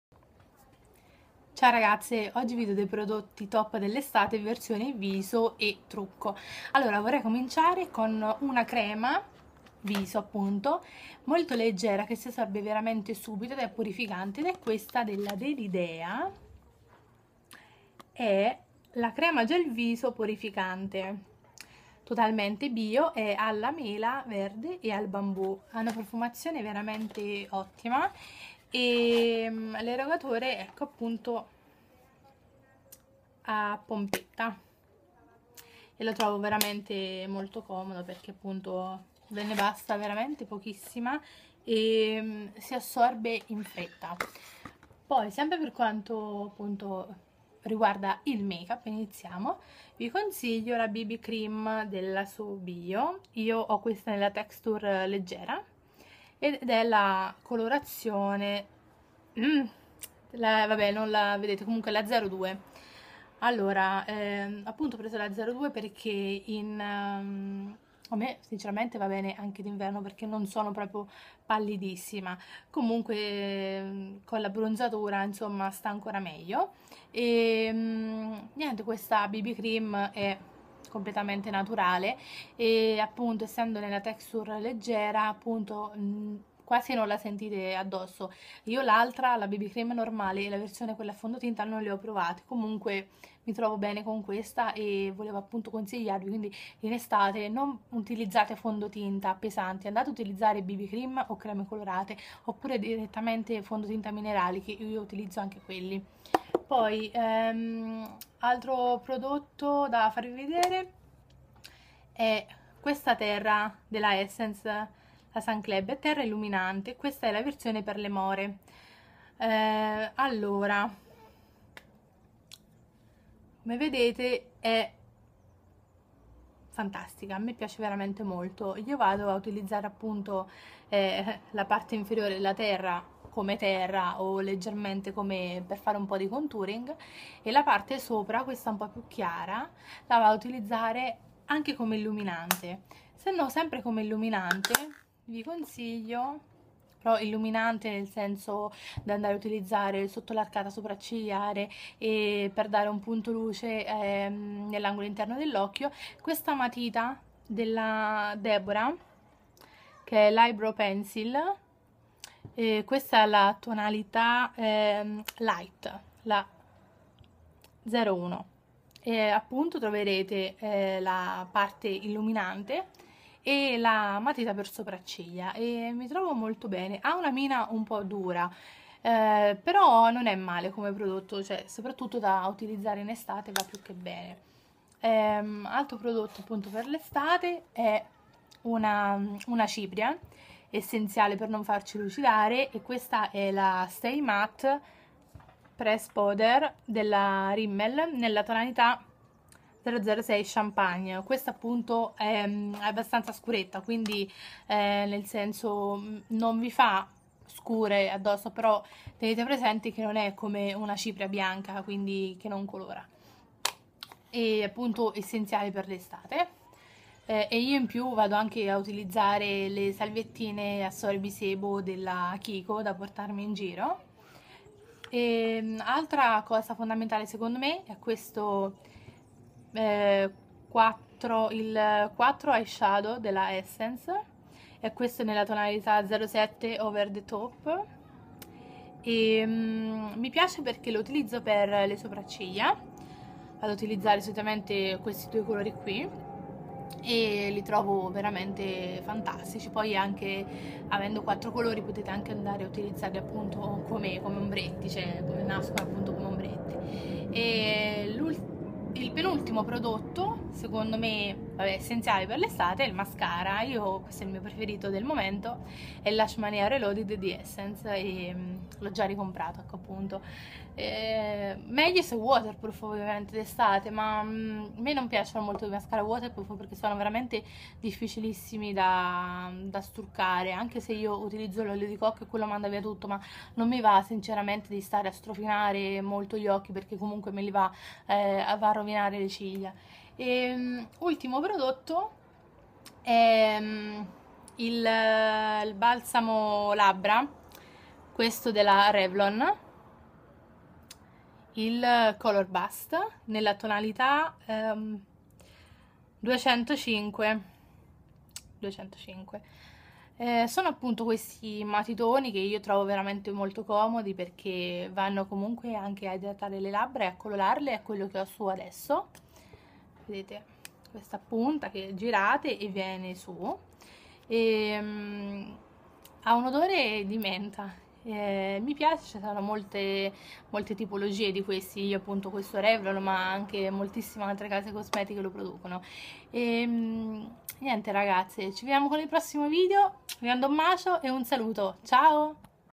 Prodotti top estate viso e trucco
Ciao ragazze! Questa è la seconda parte del video sui prodotti top dell'estate! Un bacio e a presto!